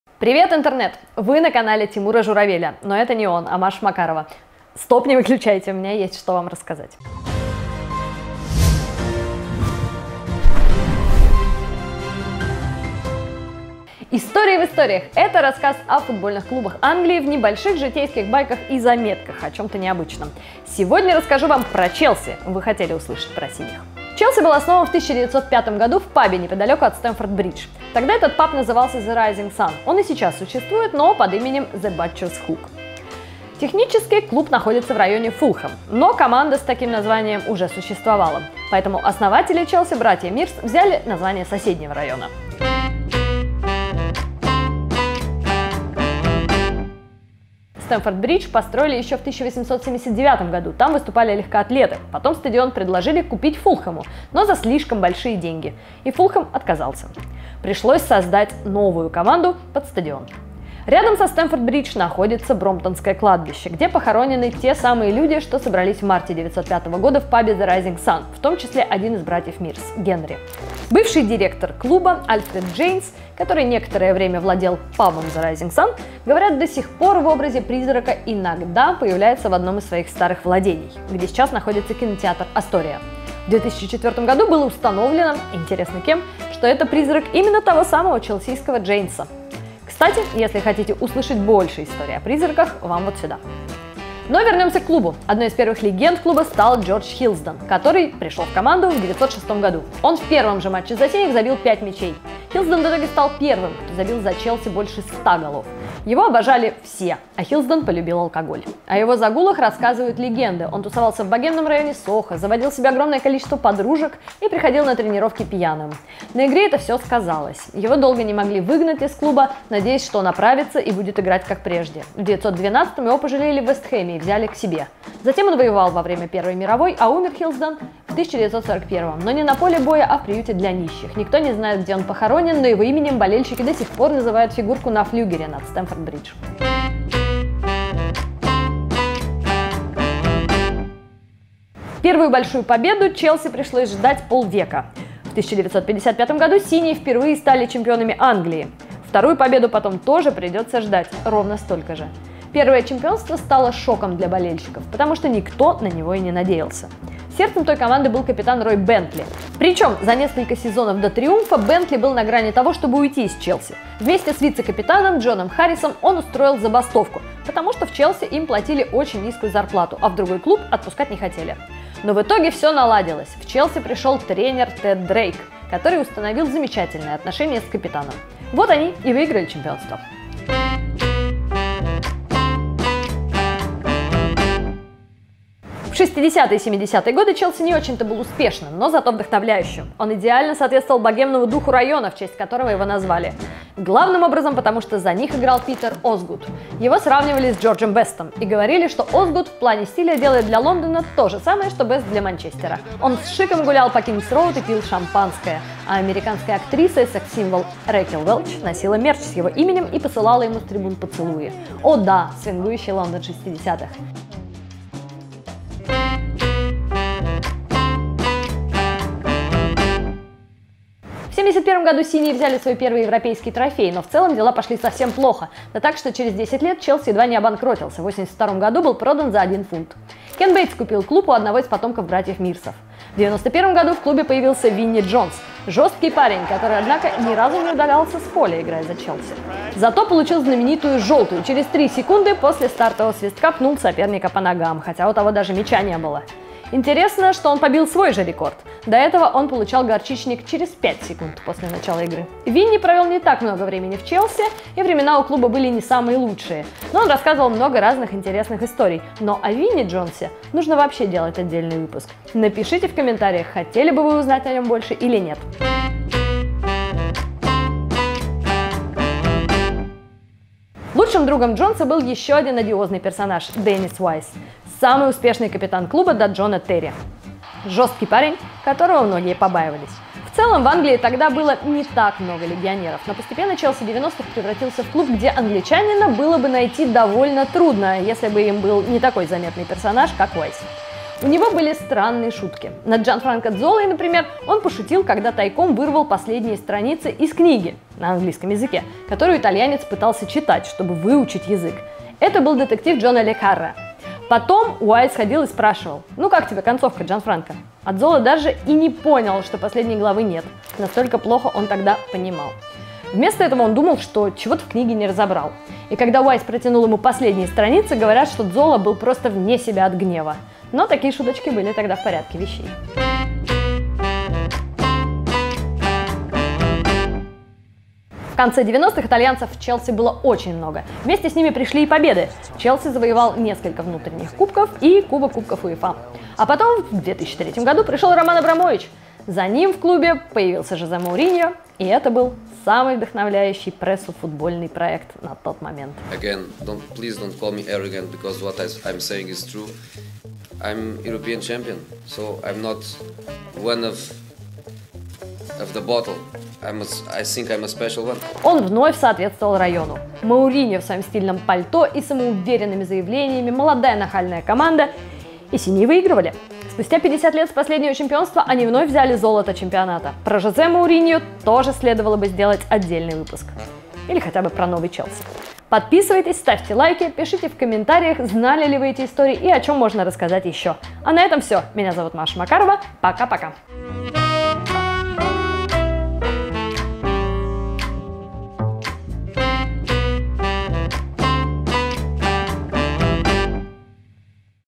Богемный район и владелец из России | Челси | История в историях #7
Челси - клуб, основанный в пабе. История флюгера над «Стэмфорд Бридж». Питер Осгуд - главный футболист «свингующего» Лондона 60-х. Новая эра Романа Абрамовича и многое другое в новом выпуске «Истории в историях».